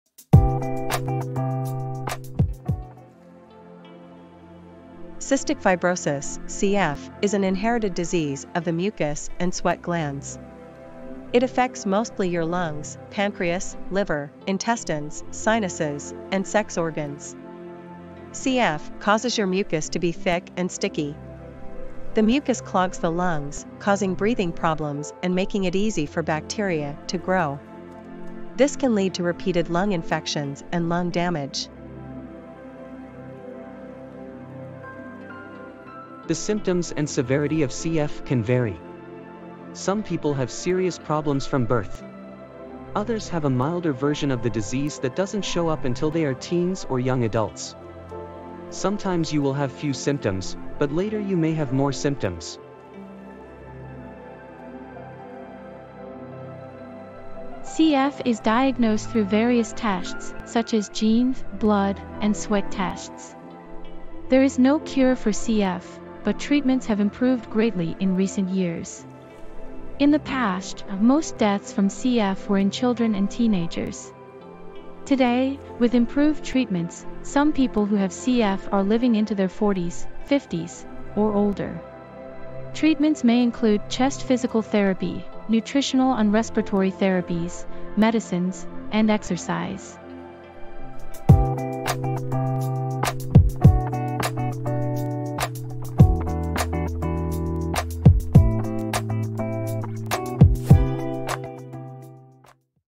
Cystic Fibrosis (CF) - Overview, Symptoms, Diagnosis, & Treatments
00:00 - Overview
00:33 - Symptoms
00:36 - Diagnosis
01:22 - Treatments

Cystic fibrosis (CF) is an inherited disease of the mucus and sweat glands. It affects mostly your lungs, pancreas, liver, intestines, sinuses, and sex organs. CF causes your mucus to be thick and sticky. The mucus clogs the lungs, causing breathing problems and making it easy for bacteria to grow. This can lead to repeated lung infections and lung damage.

The symptoms and severity of CF can vary. Some people have serious problems from birth. Others have a milder version of the disease that doesn't show up until they are teens or young adults. Sometimes you will have few symptoms, but later you may have more symptoms.

CF is diagnosed through various tests, such as gene, blood, and sweat tests. There is no cure for CF, but treatments have improved greatly in recent years. In the past, most deaths from CF were in children and teenagers. Today, with improved treatments, some people who have CF are living into their forties, fifties, or older. Treatments may include chest physical therapy, nutritional and respiratory therapies, medicines, and exercise.

NIH: National Heart, Lung, and Blood Institute